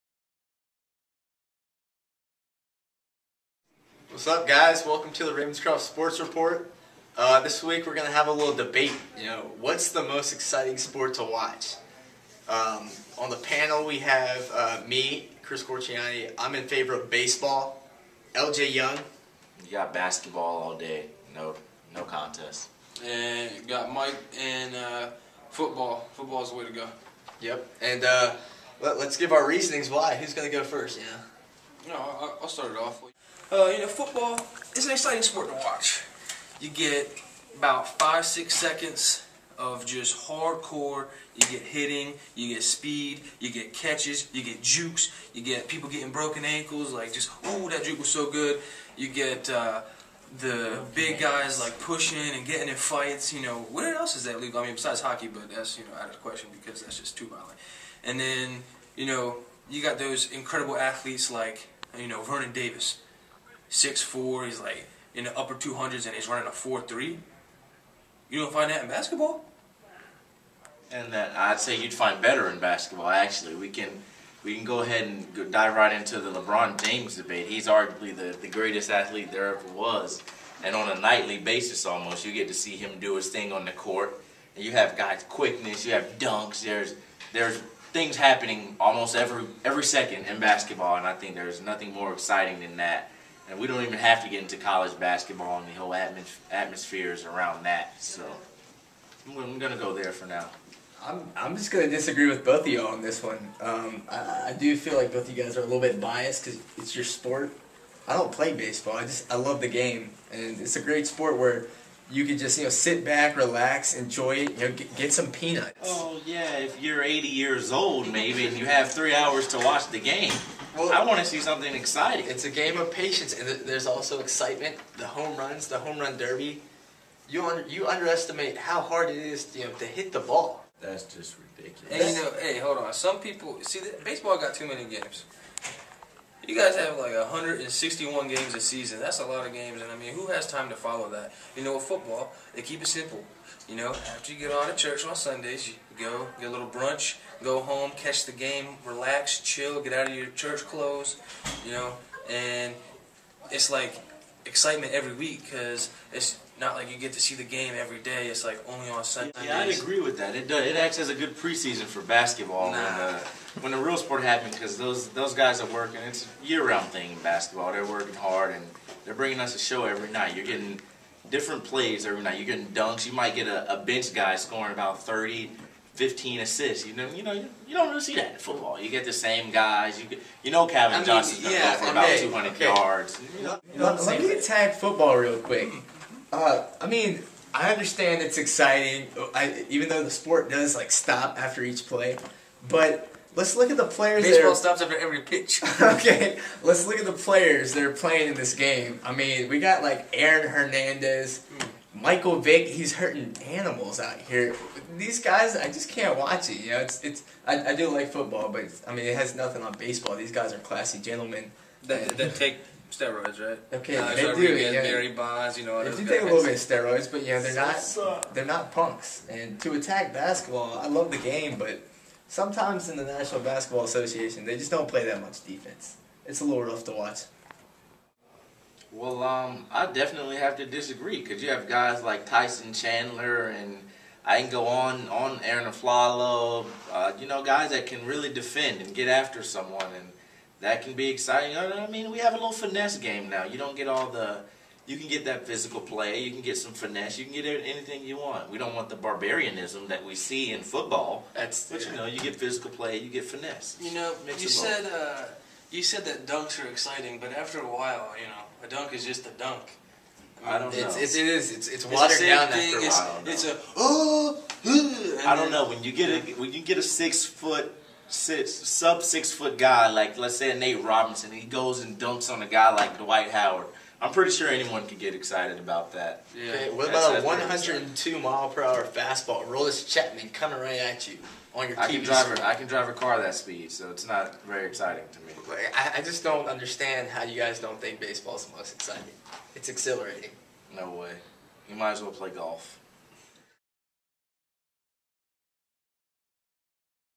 Ravenscroft Sports Report Special Edition | Most Exciting Sports Debate
Which sport is the most exciting to watch? Panelists include Chris Corchiani, LJ Young, and Mike Fagan. Camera work done by MJ Malone and Tiger Harris. Editing done by MJ Malone